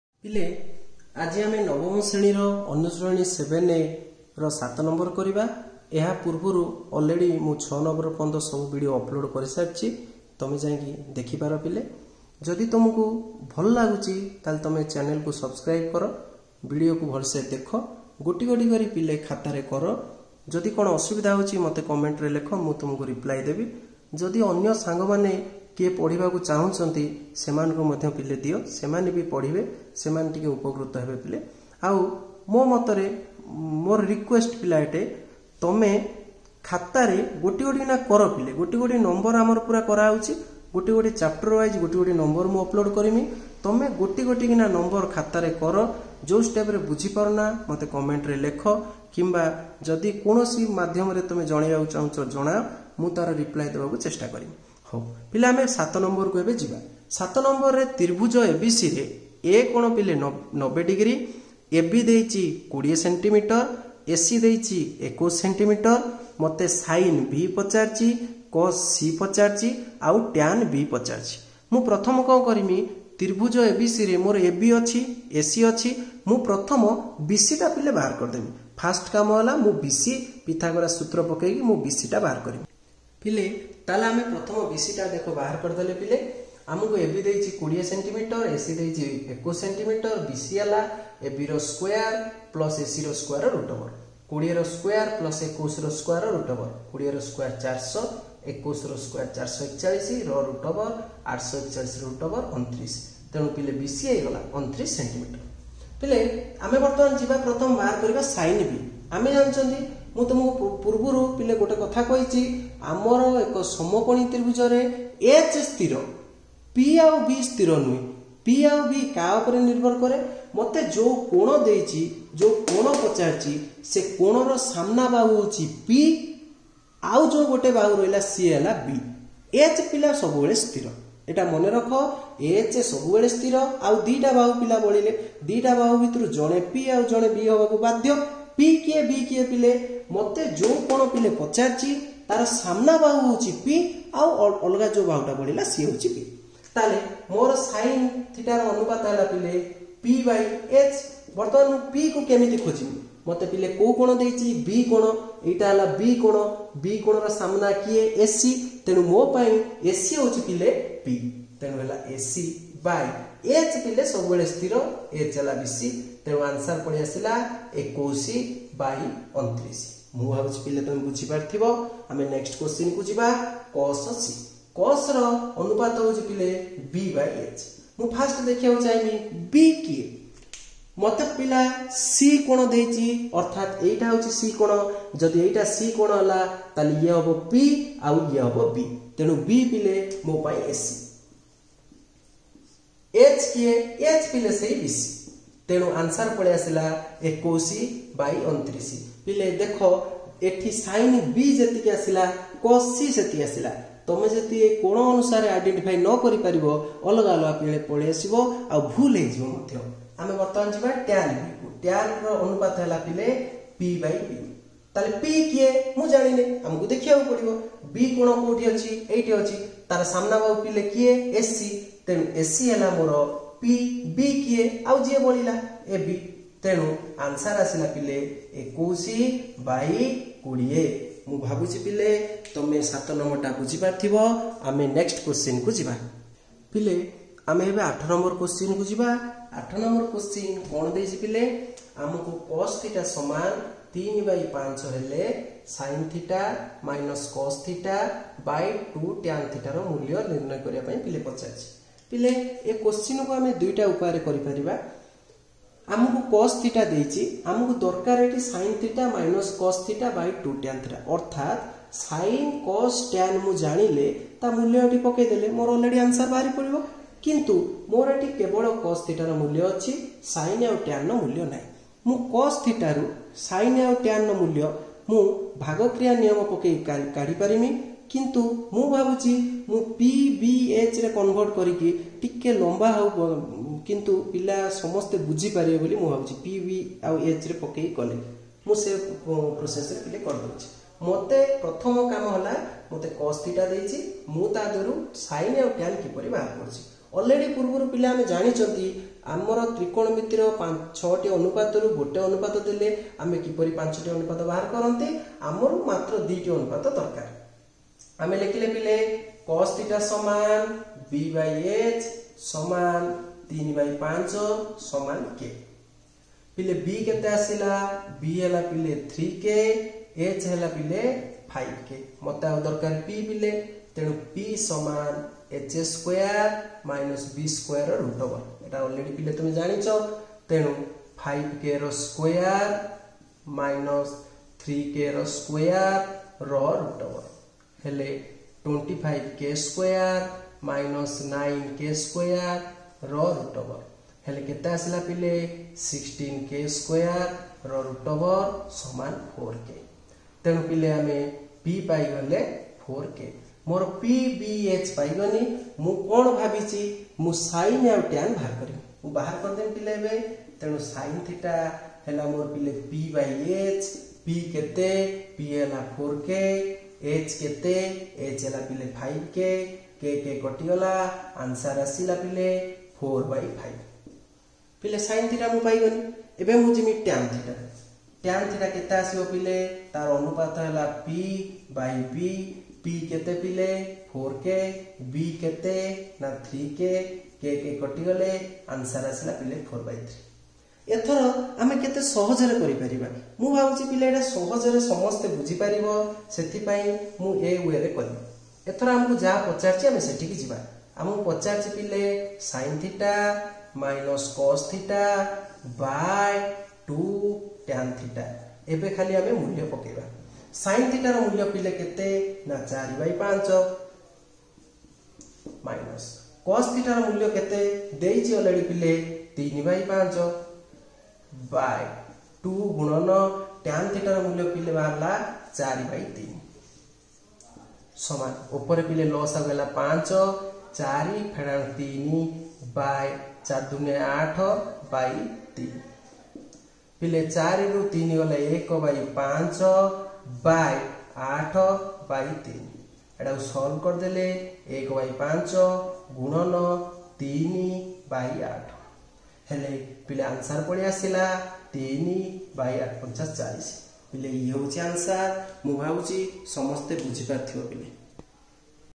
ନବମ ଶ୍ରେଣୀ/ମାଧ୍ୟମିକ ଜ୍ୟାମିତି/ତ୍ରିକୋଣମିତି/ଅନୁଶୀଳନୀ-7(a)/Q.No.-7&8/Smart School Odia
Hello friends
             you can watch class wise, chapter wise and number wise solution for odia medium student.This is my little attempt,I hope everybody will get success.